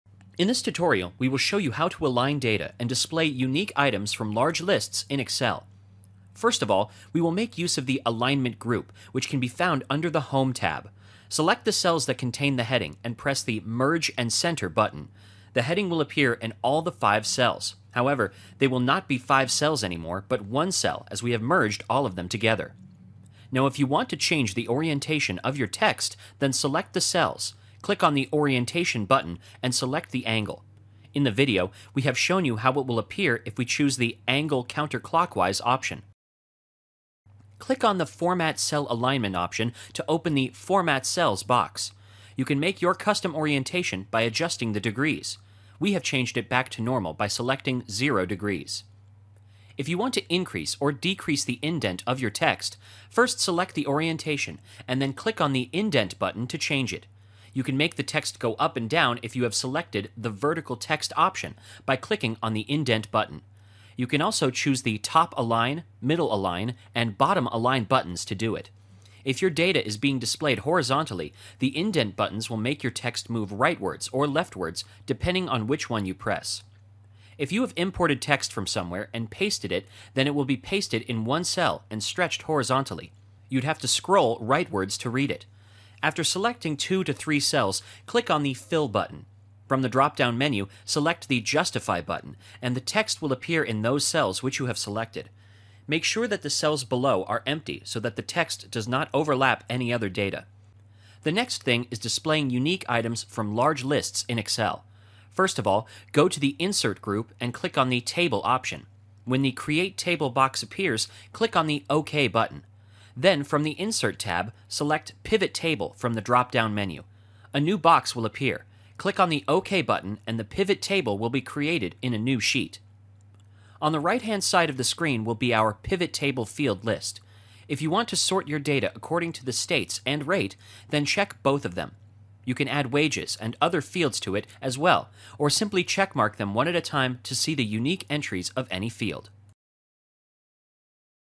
How to align data and display unique items from large lists in Excel
Learn how to change the alignment, orientation, formatting of the text in Excel. Also, learn how to find unique items with the help of pivot table.

In this tutorial, we will show you how to align data in Excel. There are several ways in which you can align data in Excel. You change the orientation of the text, format cells and increase or decrease indent. Furthermore, you can change the alignment of the text as well. We will also show you how to find unique items from large lists in Excel. Managing large lists can be a hassle. We will show you how to use a pivot table and find a solution to your problem by customizing the pivot table as per your requirements.

Step # 1 -- Apply Merge & Center
First of all, we will make use of the "alignment" group which can be found under the home tab. Select the cells that contain the heading and press the "merge and center" button. The heading will appear in all the five cells; however, they will not be five cells anymore but one cell as we have merged all of them together.

Step # 2 -- Change orientation
Now if you want to change the orientation of your text then select the cells, click on the "orientation" button and select the angle. In the tutorial, we have shown you how the text will appear if you choose the "angle counterclockwise" option.

Step # 3 -- Format Cells
Another way to do so is by clicking on the "format cell alignment" option in order to open the "format cells" box. You can make your custom orientation by adjusting the degrees; we have changed it back to normal by selecting "0" degrees.

Step # 4 -- Increase or Decrease Indent
If you want to increase or decrease the indent of your text, first select the orientation and then click on the indent button to change it. You can also choose the "top align", "middle align" and "bottom align" buttons to do it. If your data is being displayed horizontally, the indent buttons will make your text move rightwards or leftwards depending on which one you press.

Step # 5 -- Apply Justify to text
If you have imported text from somewhere and pasted it, then it will be pasted in one cell and stretched horizontally. You'd have to scroll rightwards to read it. After selecting two to three cells, click on the "fill" button. From the drop down menu, select the "justify" button and the text will appear in those cells which you have selected. Make sure that the cells below are empty so that the text does not overlap any other data. If you want your text to appear vertically, then select the cells and click on the "justify" button again. These were some of the different ways in which you can align data in Excel.

Step # 6 -- Insert Pivot Table
The next thing is displaying unique items from large lists in Excel. First of all, go to the insert group and click on the "table" option. When the "create table" box appears, click on the "ok" button. Then from the insert tab, select "pivot table" from the drop down menu. A new box will appear; click on the "ok" button and the pivot table will be created in a new sheet.

Step # 7 -- Customize Pivot Table
On the right hand side of the screen will be our "pivot table field list". If you want to sort your data according to the states and rate, then check both of them. You can add wages and other fields to it as well or simply checkmark them one at a time to see the unique entries of any field. In this manner, you can work with large lists in Excel.